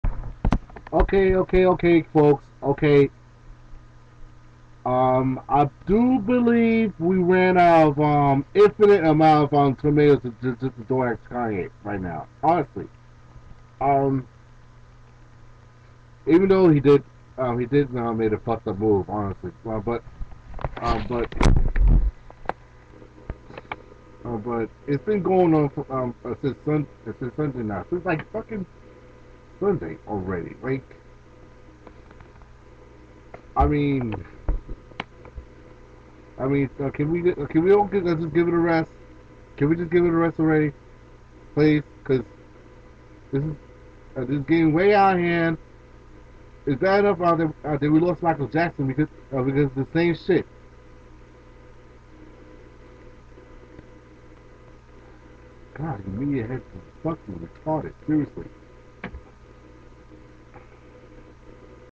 LEAVE KANYE ALONE!!!!!
I hate to be the one defending him but CAN WE ALL JUST MOVE THE FUCK ON ALREADY?!? THE MAN WAS ON JAY LENO AND PUBLICLY APOLOGIZED!!! MOVE THE FUCK ON AND GET OFF HIS DICK! smh. FollowUs at Soundcloud.com/heartlessn8tion.